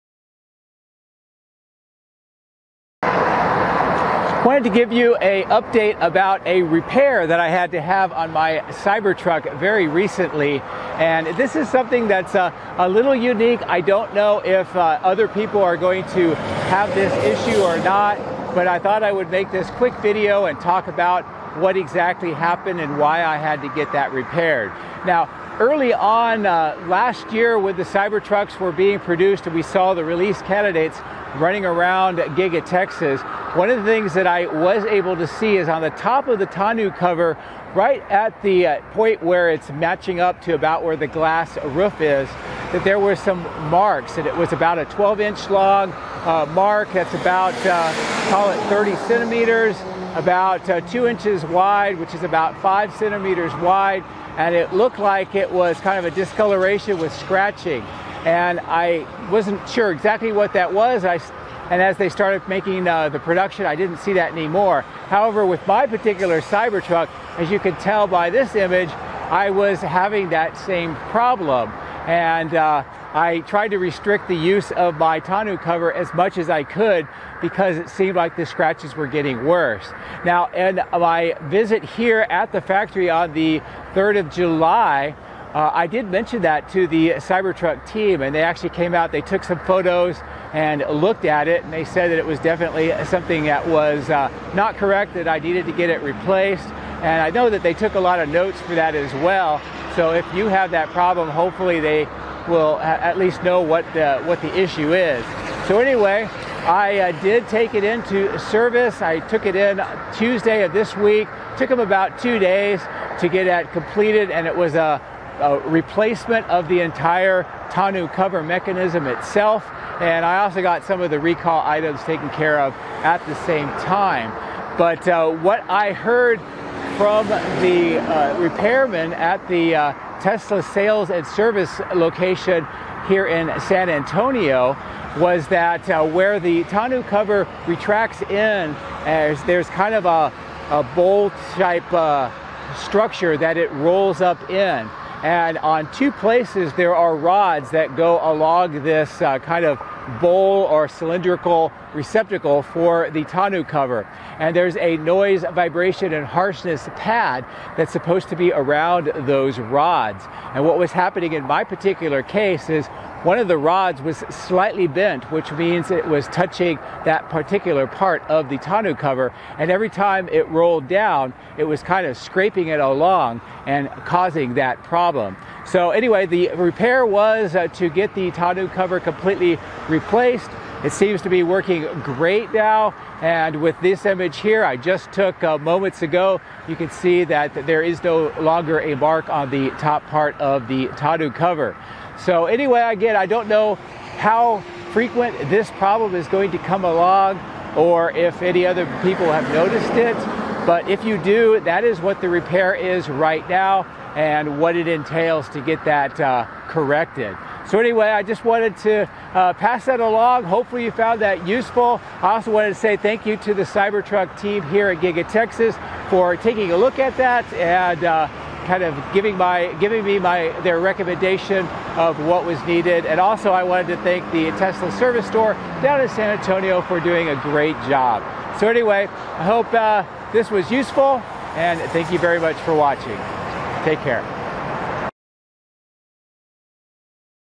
Cybertruck Tonneau Cover Mechanism failure & replacement
I recently had my Cybertruck Tonneau cover replaced (under warranty) because of a mechanical issue which was causing deep scratches & a 5 Cm by 30 Cm mark on the top center of the cover.

Mechanically, the cover worked and operated correctly, but the marring of the surface was getting noticeably worse, so I essentially stopped using it while I figured out the repair.

As it happened, I was at the Giga Texas factory for a tour in early July, & while I was there, I talked to some members of the Cybertruck team & I mentioned this issue. I also mentioned I had noticed similar issues with early Release Candidate (RC) versions of the Cybertruck around the factory & that I thought it might be related.

They looked at my cover, took images & measurements. They told me I should get a service appointment & have the entire cover replaced. So, I set up an appointment with my Tesla Service Center in San Antonio & recently had the work completed.

The problem turned out to be one of the rods that run along the width of the cover & is part of the retracting mechanism. There is supposed to be a NVH (Noise, Vibration & Harshness) pad to prevent this from happening, but apparently one of these rods had a bend in it & this was causing the scraping and delamination as the cover retracted and then extended.

For illustration, I used a few video clips form Munro Live when they disassembled the Tonneau cover as part of their Cybertruck series of videos.

The solution was a complete Tonneau cover replacement, which was done (under warranty) & now it seems to be rectified.

I am not sure how many might have a similar issue, but I hope you find this video helpful.